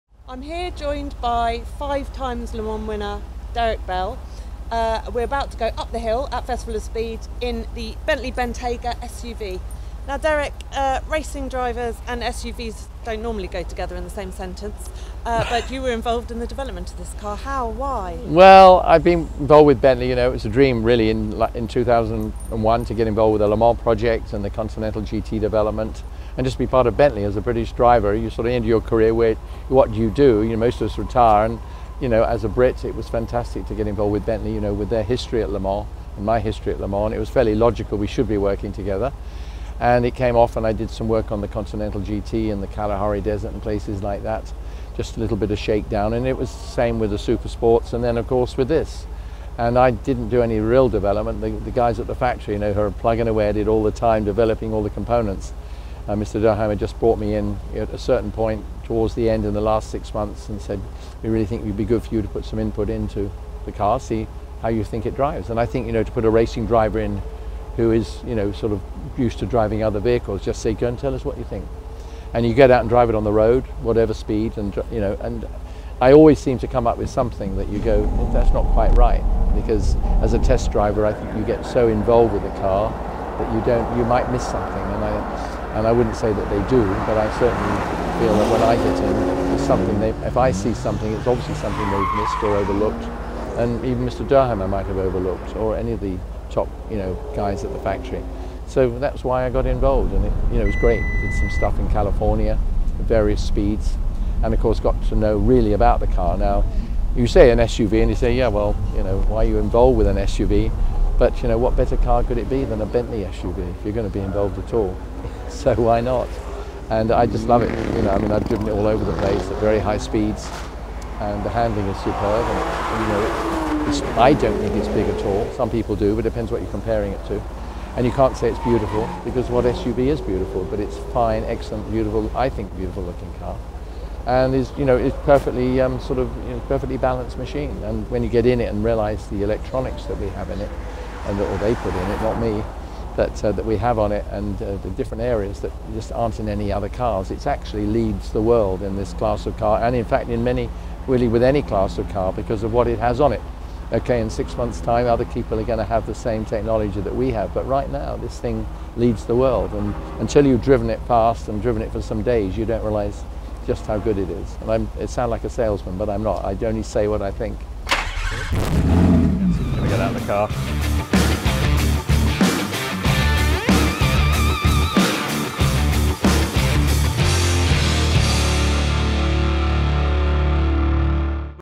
5x Le Mans Winner Derek Bell Shows Off the Bentley Bentayga!
5x Le Mans Winner & Global Bentley Ambassador Derek Bell talked to Goodwood about the role he had in developing the new Bentley Bentayga and his desire to be a part of the Bentley story, before taking the SUV up the Hillclimb!